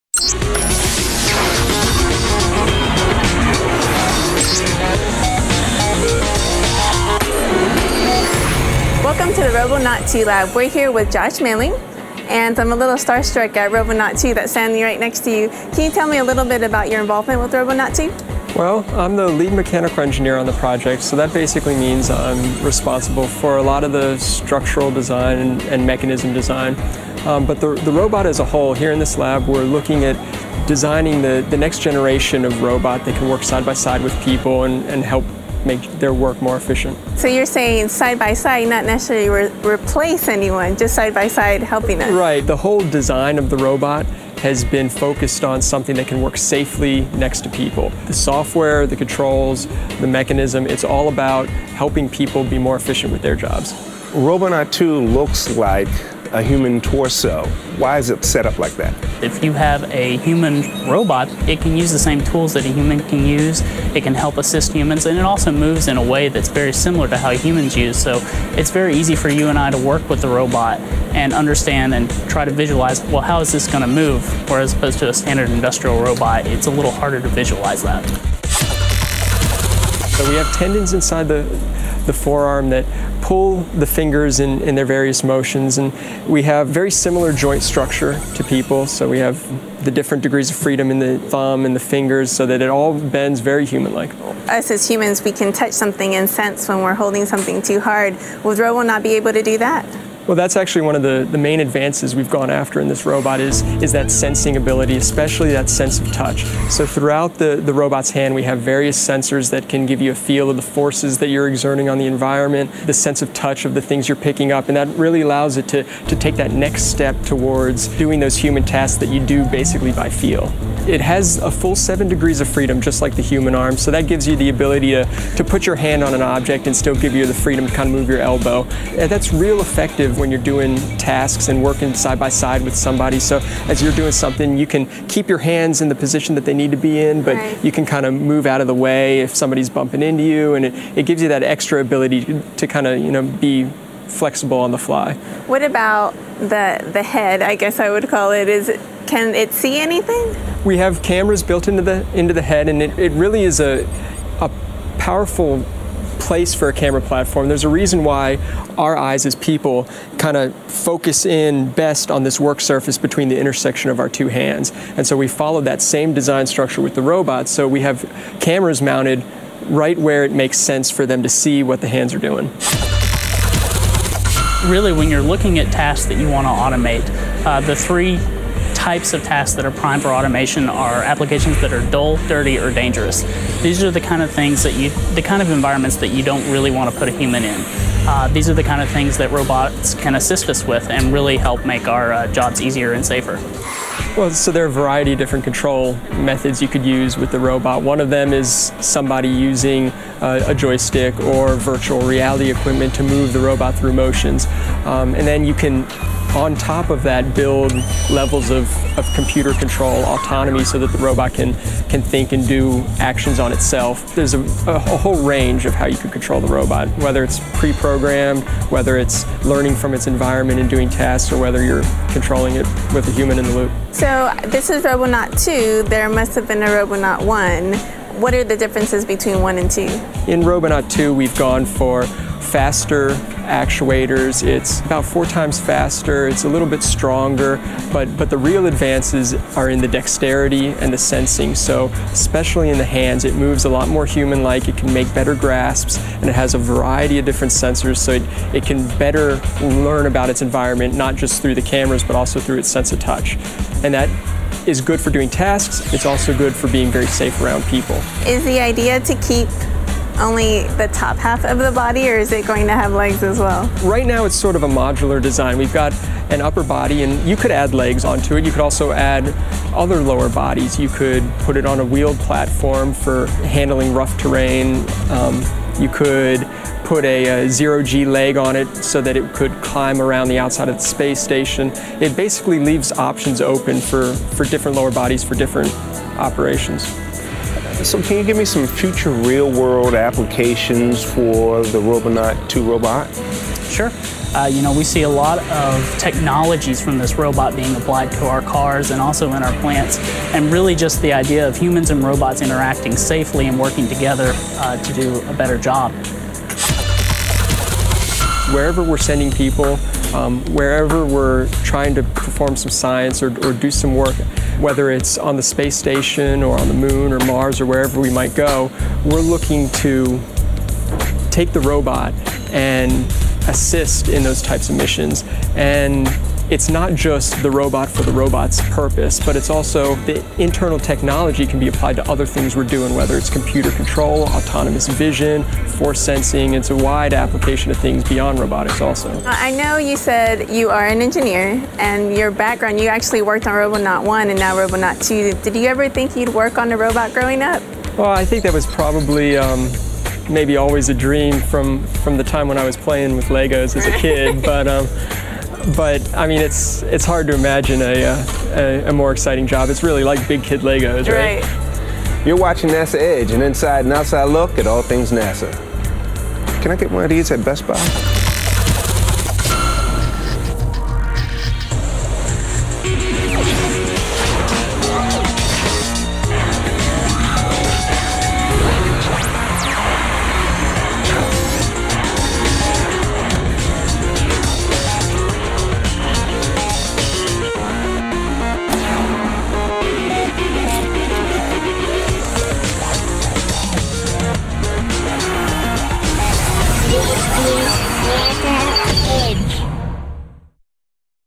First humanoid robot in space | On the International Space Station by 2011
Video courtesy of NASA